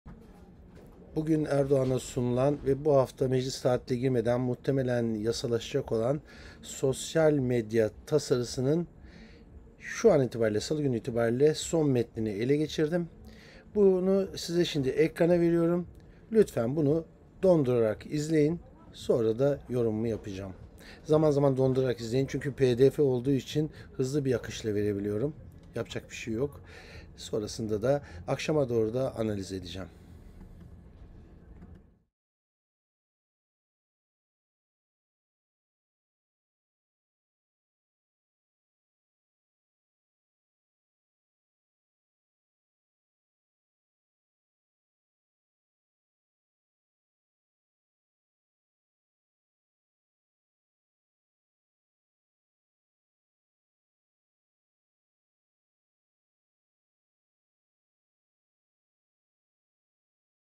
İŞTE SOSYAL MEDYA YASA TEKLİFİNİN SON HALİNİN TAM METNİ.TWITTER, FACEBOOK,YOUTUBE NASIL ETKİLENECEK!
ÖZEL HABER